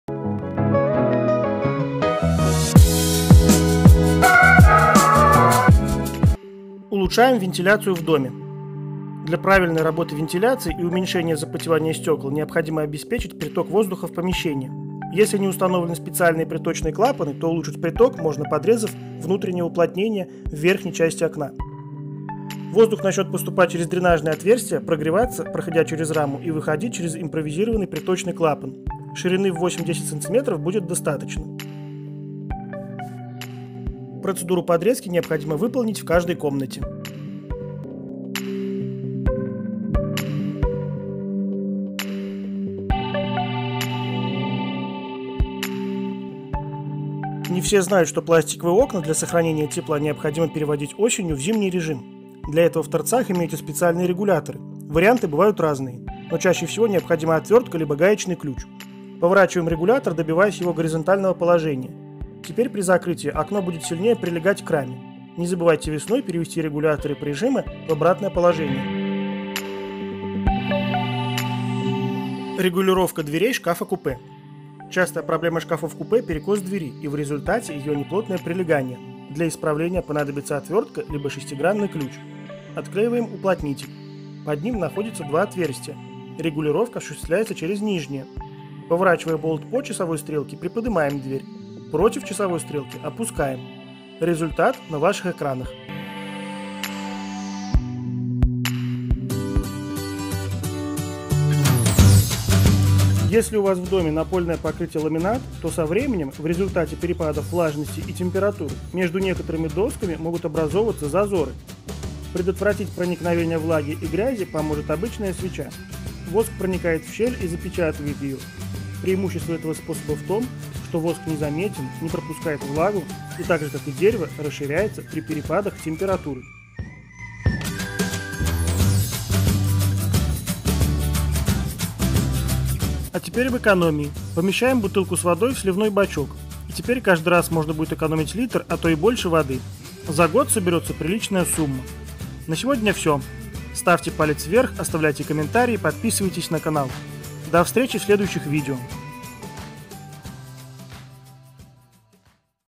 ПЯТЬ РАБОТАЮЩИХ лайфхаков для нашего жилья
Квартира и дом. Как быстро улучшить наше жильё? Простые и эффективные способы:
   +Улучшаем вентиляцию жилья
   +Переводим окна в зимний режим
   +Продлить срок службы ламината
   +Регулировка двери шкафа-купе
   +Экономим воду в туалете 😏👍😃

ВЕСЬ КОНТЕНТ КАНАЛА: ⬇️
All channel content: